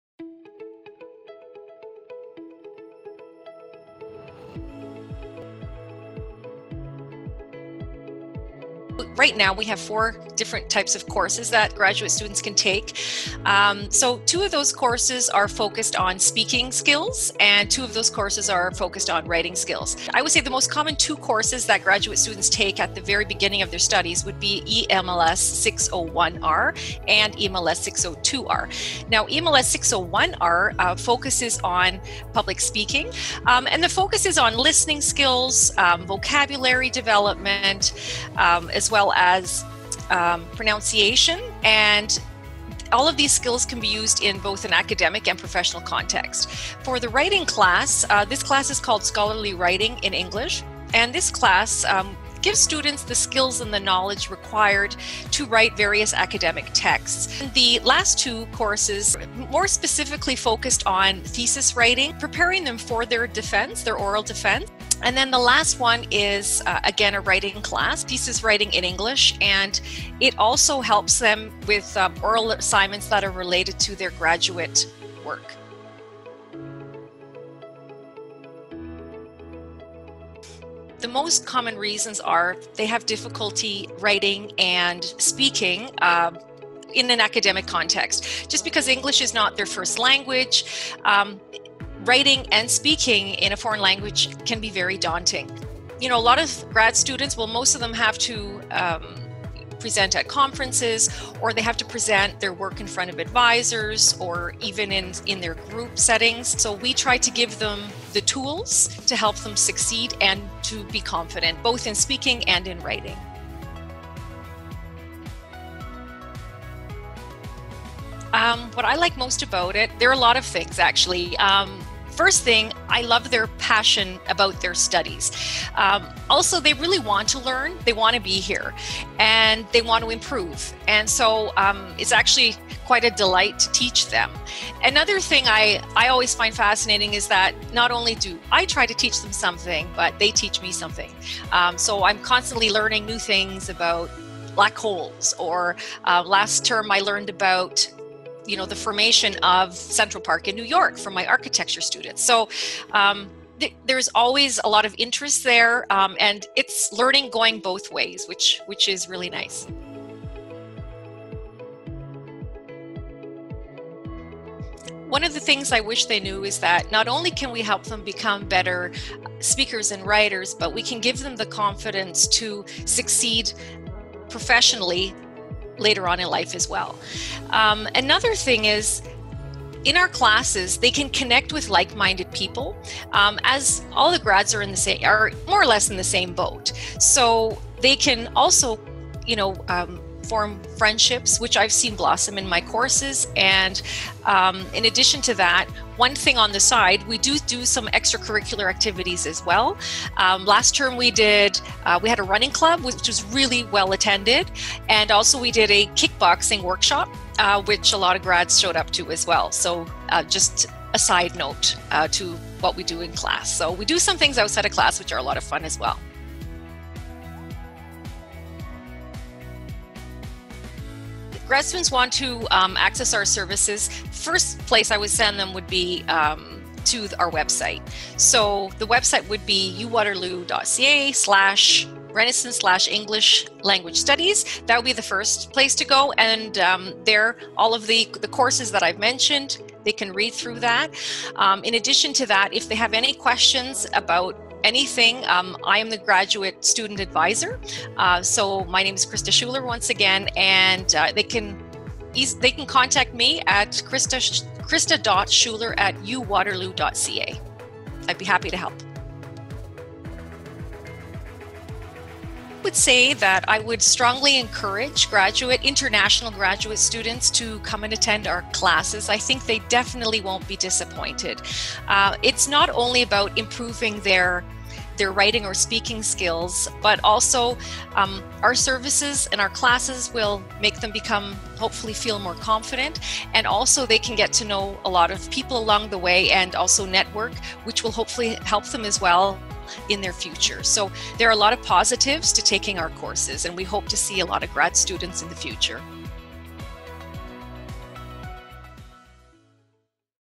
English Language Studies: Graduate Studies Services Fair
Christa Schuller, English Language Studies Graduate Advisor explains how ELS offers courses on:
• clear and effective writing
• accurate and fluent speaking
• presentation skills development
• thesis and scholarly article writing
• undergraduate teaching, thesis defense, and conference presentations